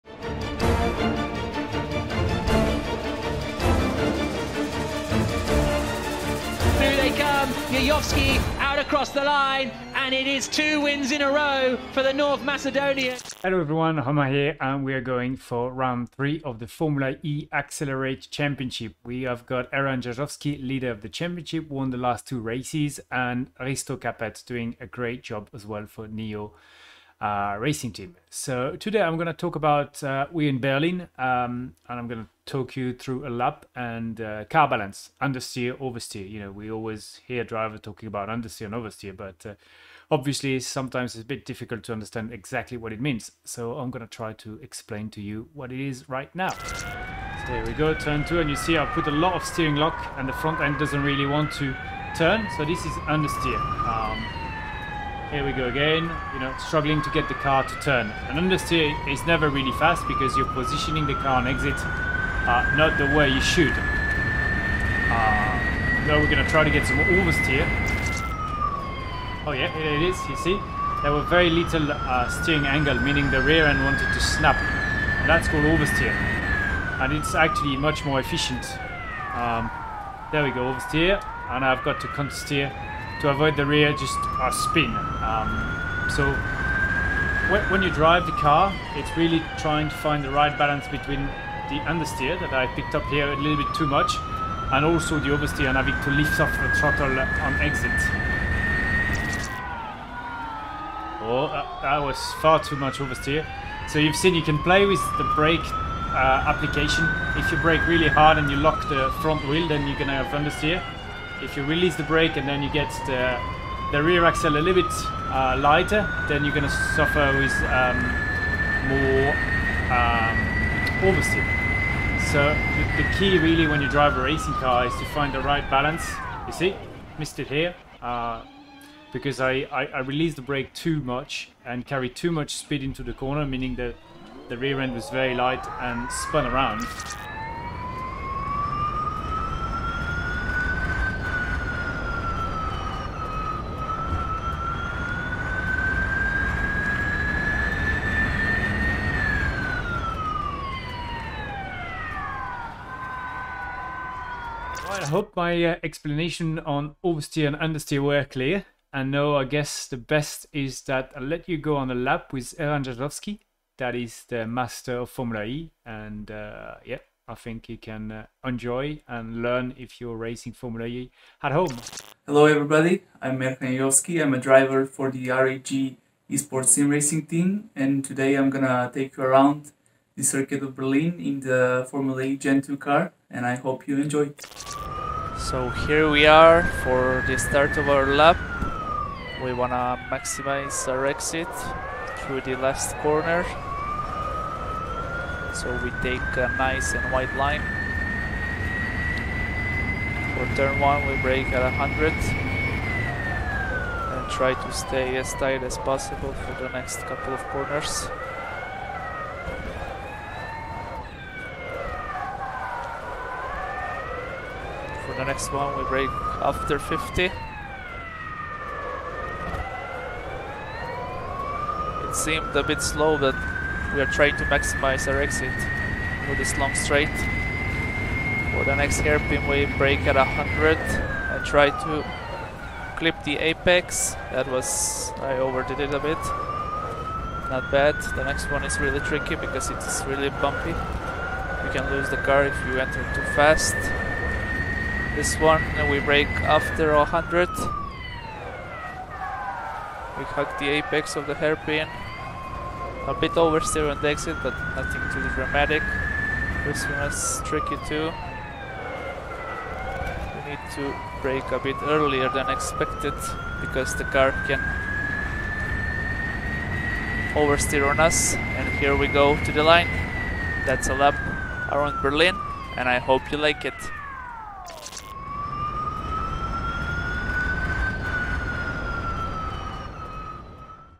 PREVIEW / Formula E Accelerate Round 3 in Berlin 🇩🇪
I explain what car balance is: oversteer vs understeer.
Erhan Jajovski drives a lap around Berlin.